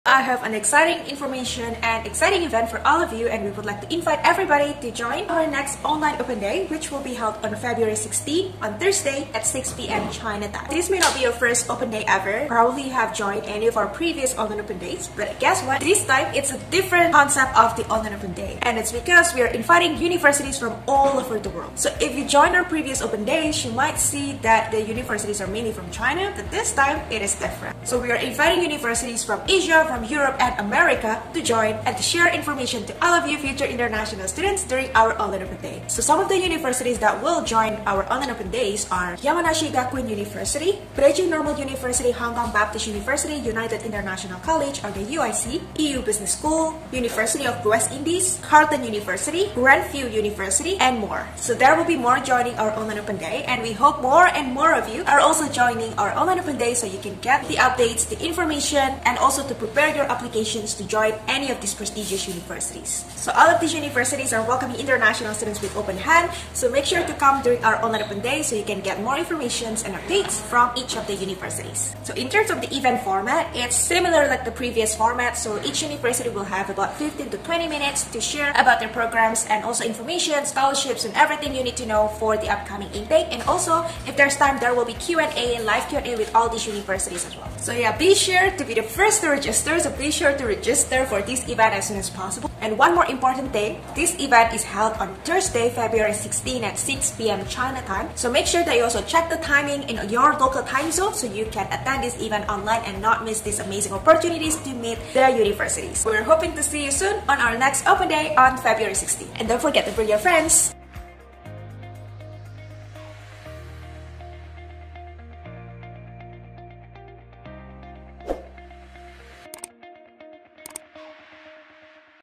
Meet Top Universities Worldwide on Thursday, 16 February!
Are you interested in studying abroad and applying to a university for 2023 intake?
Global Admissions is inviting you to join our upcoming Open Day to meet some of the top universities around the world.
The online fair will start on Thursday, 16th February 2023 at 6:00 PM Beijing time.
Each university will give a presentation about their university and programs.
After their presentation, you will have the opportunity to connect with the universities to ask them questions directly.

Podcast where we share useful information for students like you!